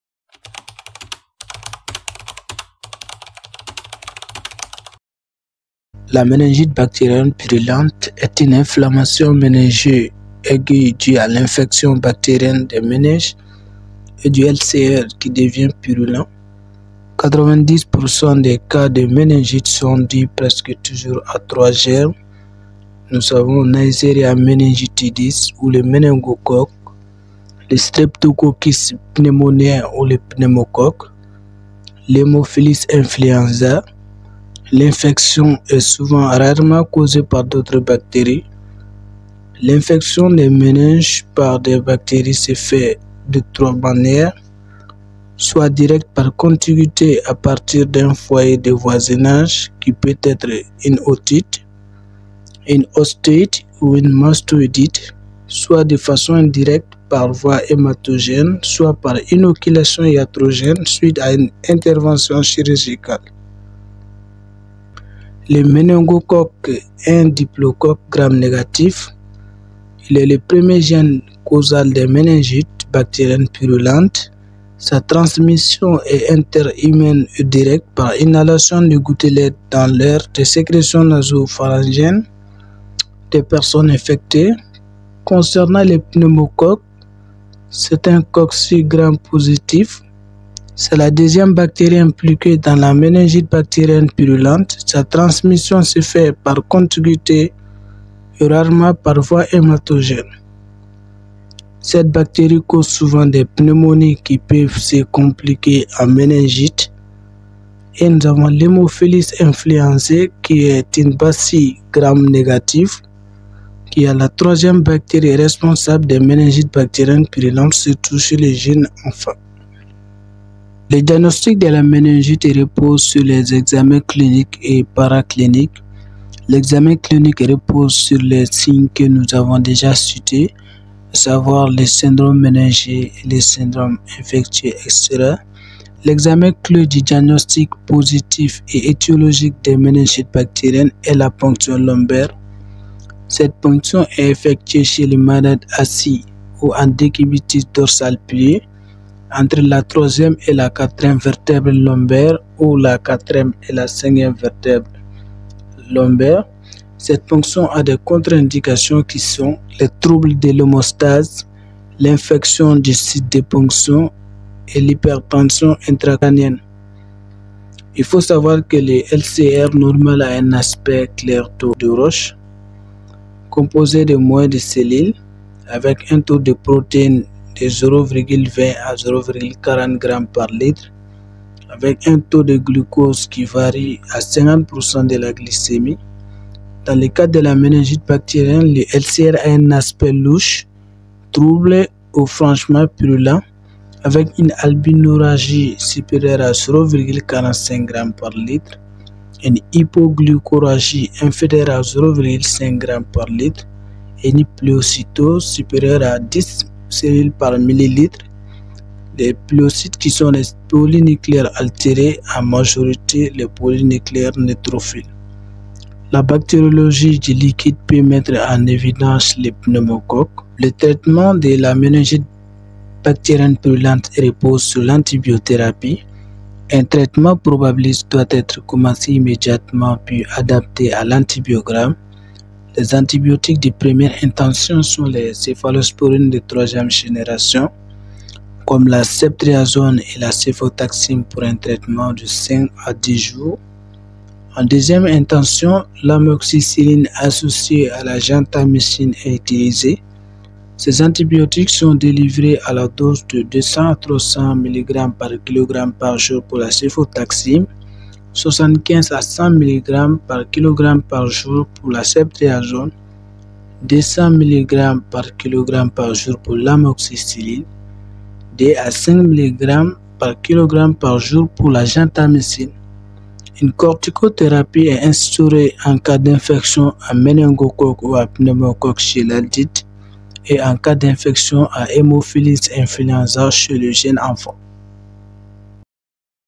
Méningite bactérienne purulente
La méningite bactérienne purulente  est une inflammation méningée aiguë due à une infection bactérienne des méninges et du LCR, qui devient purulent.
80% des cas de méningites sont dues presque toujours à 3 germes :
         Neisseria meningitidis ou méningocoque
         Streptococcus pneumoniae ou pneumocoque
         Haemophilus influenza
L’infection est souvent rarement causée par d’autres bactéries.
L’infection des méninges par ces bactéries se fait de trois manières, soit direct par contiguïté à partir d’un foyer de voisinage qui peut être une otite, une ostéite, une mastoïdite etc.., soit de façon indirecte par voie hématogène, soit par inoculation iatrogène suite à une intervention chirurgicale
Le méningocoque est un diplocoque gram négatif.  Il est le premier germe causal des méningites bactériennes purulentes.
Sa transmission est inter humaine et directe par inhalation de gouttelettes dans l'air des sécrétions naso-pharyngiennes des personnes infectées.
Concernant le pneumocoque, c’est un Cocci gram positif. C’est la deuxième bactérie impliquée dans la méningite bactérienne purulente.
Sa transmission se fait par contiguïté et rarement par voie hématogène
Cette bactérie cause souvent des pneumonies qui peuvent se compliquer en méningite.
L’hemophilis influenzae qui est une bacille gram négatif est la troisième bactérie responsable des méningites bactriennes purulente, surtout chez le jeune enfant.